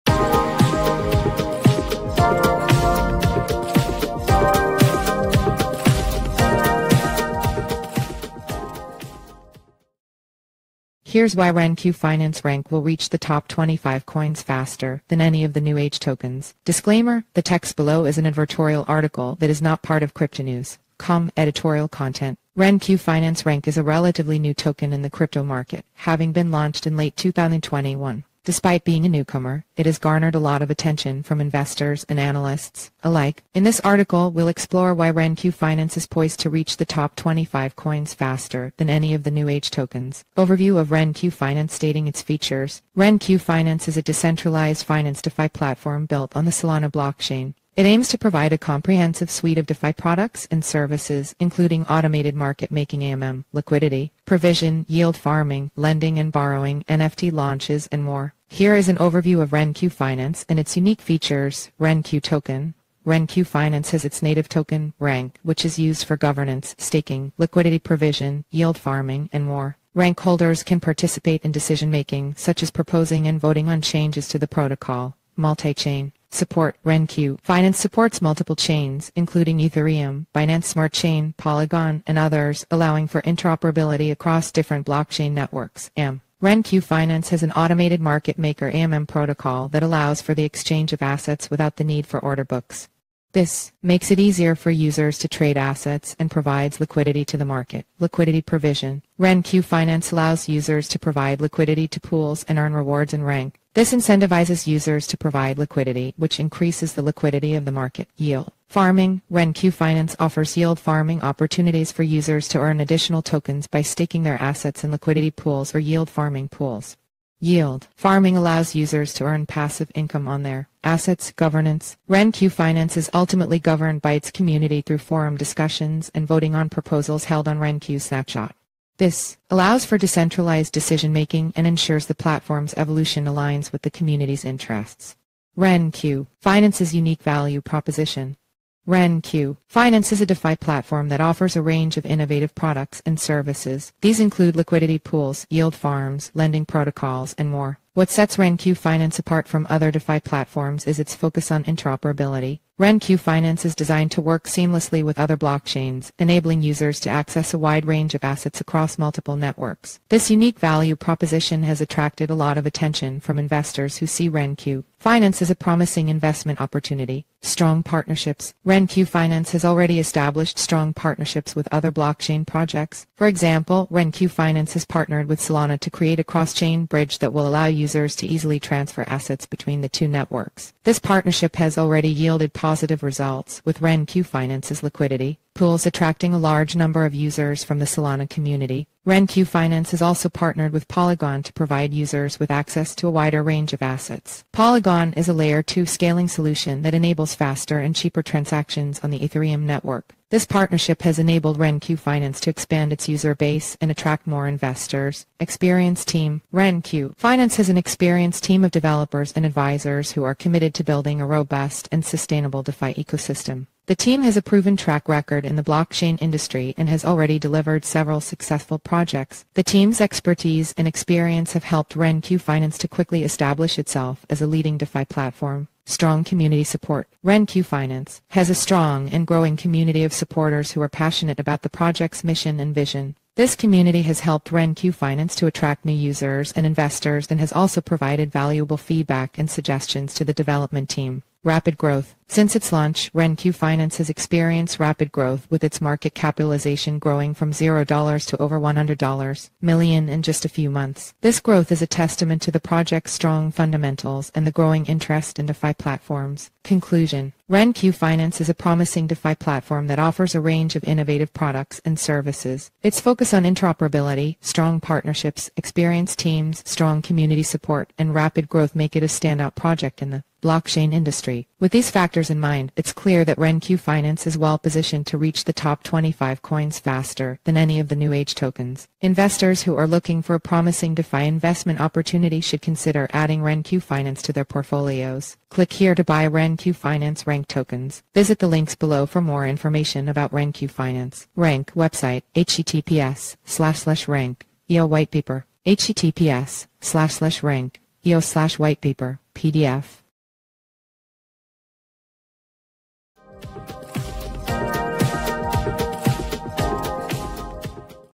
Here's why RenQ Finance (RENQ) will reach the top 25 coins faster than any of the new-age tokens
RenQ Finance (RENQ) is a relatively new token in the crypto market, having been launched in late 2021. Despite being a newcomer, it has garnered a lot of attention from investors and analysts alike.

In this article, we'll explore why RenQ Finance is poised to reach the top 25 coins faster than any of the new-age tokens.

Overview of RenQ Finance stating its features.

RenQ Finance is a decentralized finance (DeFi) platform built on the Solana blockchain. It aims to provide a comprehensive suite of DeFi products and services, including automated market making (AMM), liquidity provision, yield farming, lending and borrowing, NFT launches, and more. Here is an overview of RenQ Finance and its unique features:

RenQ Token: RenQ Finance has its native token, RENQ, which is used for governance, staking, liquidity provision, yield farming, and more. RENQ holders can participate in decision-making, such as proposing and voting on changes to the protocol.Multi-chain support: RenQ Finance supports multiple chains, including Ethereum, Binance Smart Chain, Polygon, and others, allowing for interoperability across different blockchain networks.AMM: RenQ Finance has an automated market maker (AMM) protocol that allows for the exchange of assets without the need for order books. This makes it easier for users to trade assets and provides liquidity to the market.Liquidity provision: RenQ Finance allows users to provide liquidity to pools and earn rewards in RENQ. This incentivizes users to provide liquidity, which increases the liquidity of the market.Yield farming: RenQ Finance offers yield farming opportunities for users to earn additional tokens by staking their assets in liquidity pools or yield farming pools. Yield farming allows users to earn passive income on their assets.Governance: RenQ Finance is ultimately governed by its community through forum discussions and voting on proposals held on RenQ's Snapshot. This allows for decentralized decision-making and ensures the platform's evolution aligns with the community's interests.

RenQ Finance's Unique Value Proposition.

RenQ Finance is a DeFi platform that offers a range of innovative products and services. These include liquidity pools, yield farms, lending protocols, and more.

What sets RenQ Finance apart from other DeFi platforms is its focus on interoperability. RenQ Finance is designed to work seamlessly with other blockchains, enabling users to access a wide range of assets across multiple networks. This unique value proposition has attracted a lot of attention from investors who see RenQ Finance as a promising investment opportunity.

Strong Partnerships.

RenQ Finance has already established strong partnerships with other blockchain projects. For example, RenQ Finance has partnered with Solana to create a cross-chain bridge that will allow users to easily transfer assets between the two networks. This partnership has already yielded positive results, with RenQ Finance's liquidity pools attracting a large number of users from the Solana community.

RenQ Finance has also partnered with Polygon to provide users with access to a wider range of assets. Polygon is a Layer 2 scaling solution that enables faster and cheaper transactions on the Ethereum network. This partnership has...